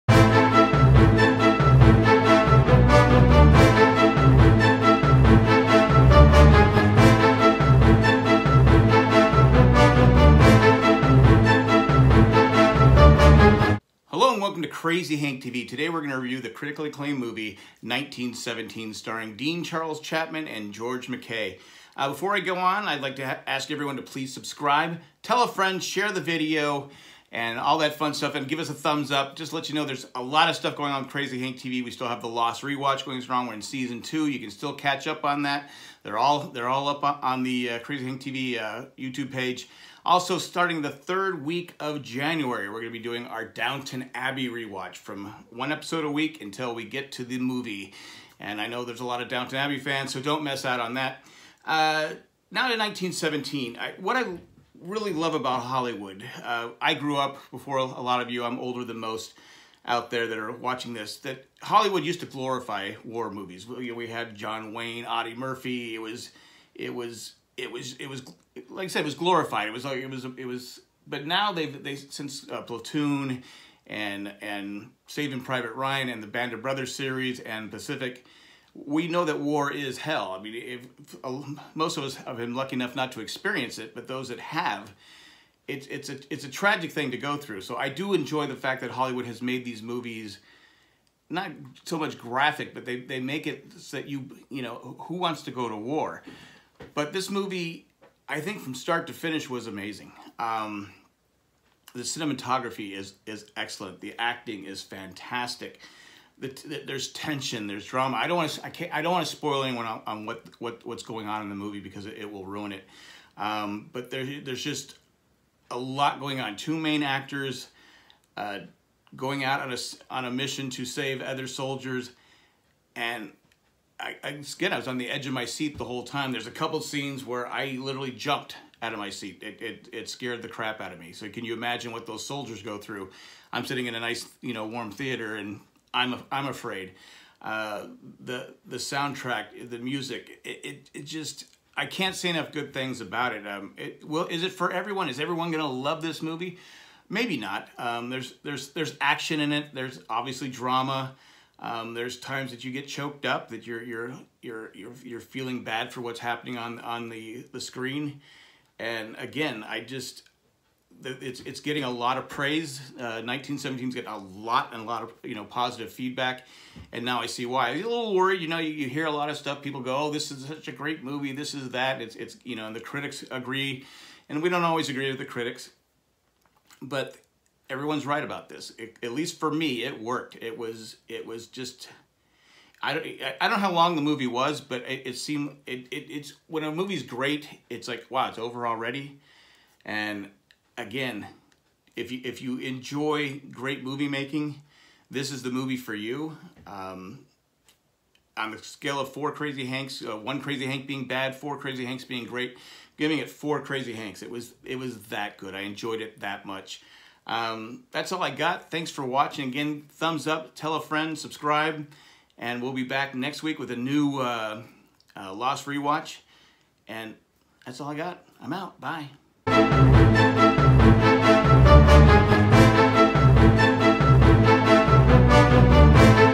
1917 Movie Review
On this episode of Crazy Hank TV we review the new movie 1917. Starring Dean-Charles Chapman, George MacKay, Daniel Mays, Colin Firth, Andrew Scott, and Benedict Cumberbatch.